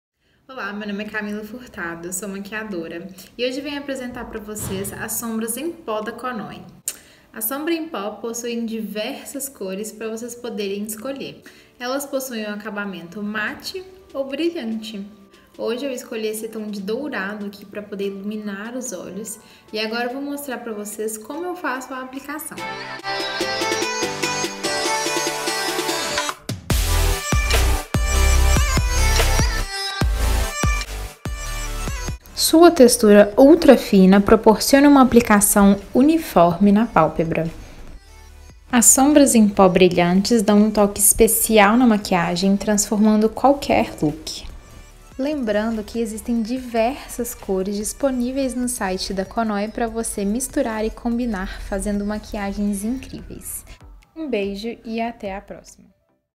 RESENHA SOMBRA EM PÓ - POR CAMILA FURTADO
A @maria.milla testou a nossa sombra em pó e mostra como sua pigmentação é ótima!